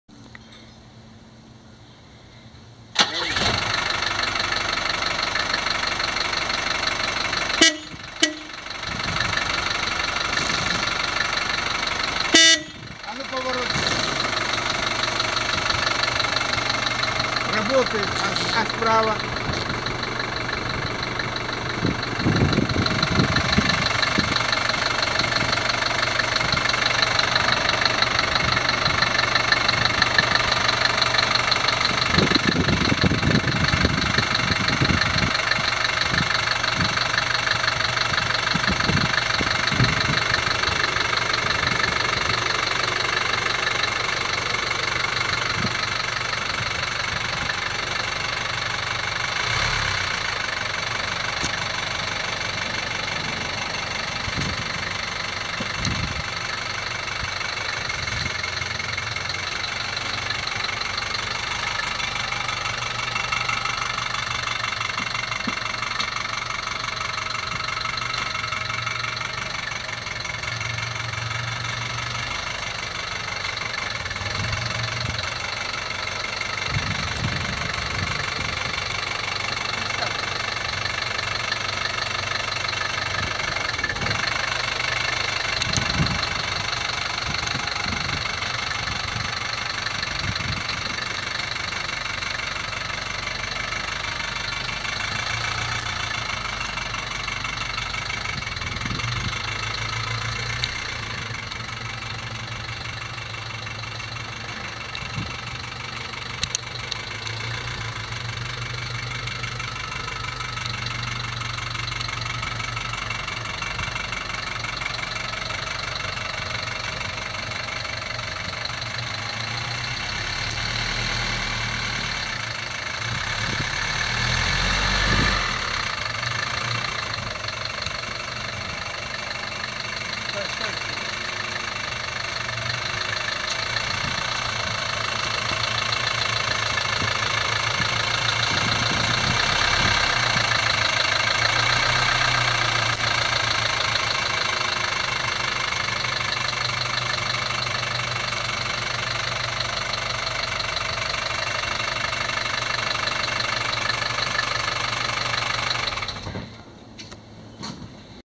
Checking electronics on 1988 Mercedes Gelande Wagen Wolf
1988 Military Mercedes G-Wagen Volf, everything works good!